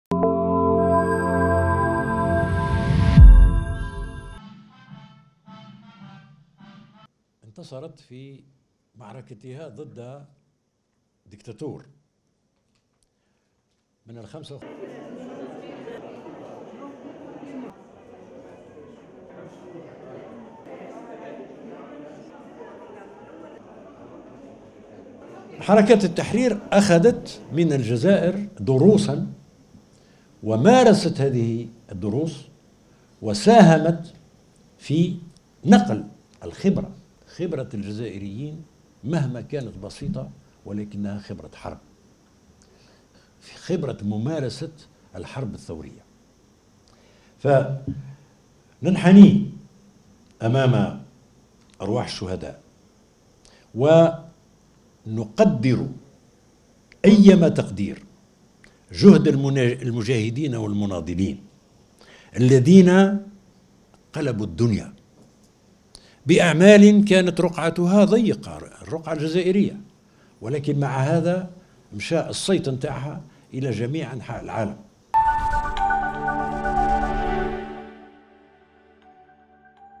دور الجزائر في دعم الحركات التحررية عبر العالم محور ندوة بمقر الأرشيف الوطني
الجزائر (وأج) -  أبرز متدخلون في ندوة تاريخية احتضنها مقر الأرشيف الوطني (الجزائر العاصمة), الدور الفعال الذي لعبته الجزائر في دعم حركات التحرر وتصفية الاستعمار في افريقيا وفي باقي مناطق العالم.